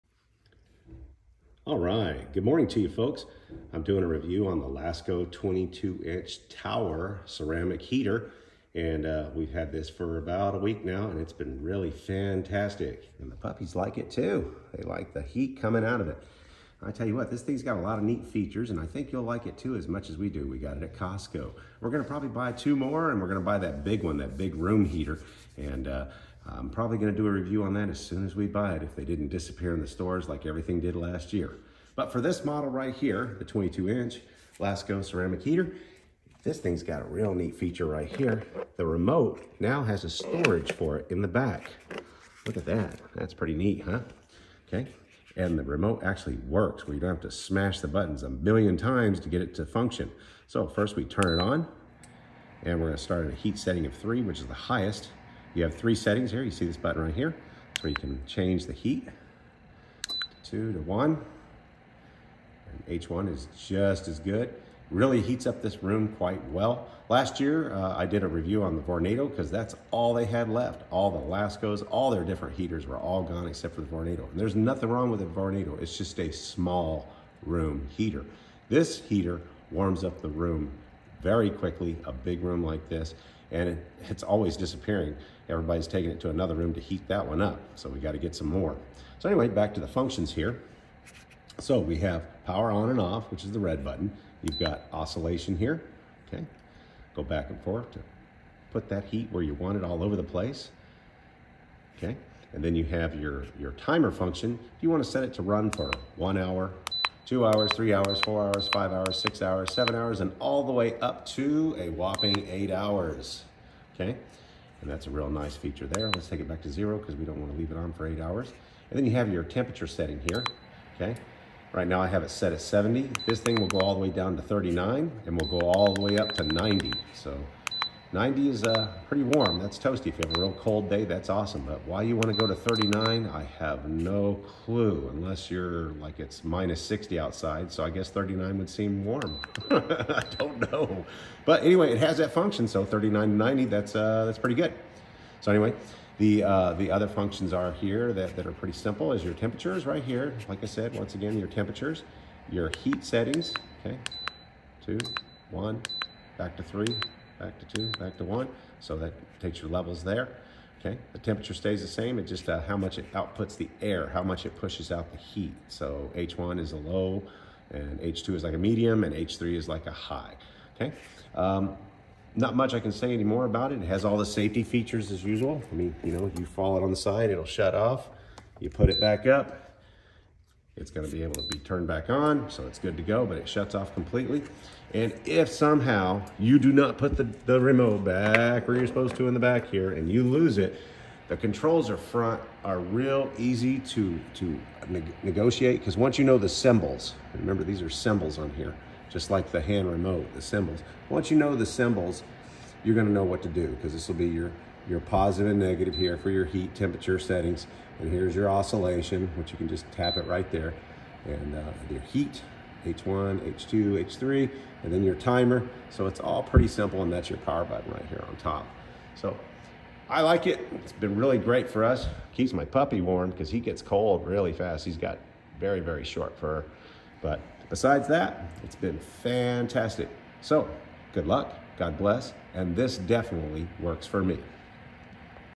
👀🔥 Lasko 22 Inch Ceramic Tower Heater-😲💥👍 Review - We luv it !!! Great heater Great remote!!! 😇❄
REVIEW -Fantastic Heater👍😲 !! Lasko 22 Inch Ceramic Tower Heater Item 1415865 Model CT22445
Features:
3-Speed Heat Settings
Digital Controls with Touch Sensing Technology
8-Hour Auto Shut-Off Timer
25% Wider Oscillation
Remote Control
health healthylifestyle wellness stayhealthy fitness fitnessjourney healthcoach fitnessfreak bodyfitness 60.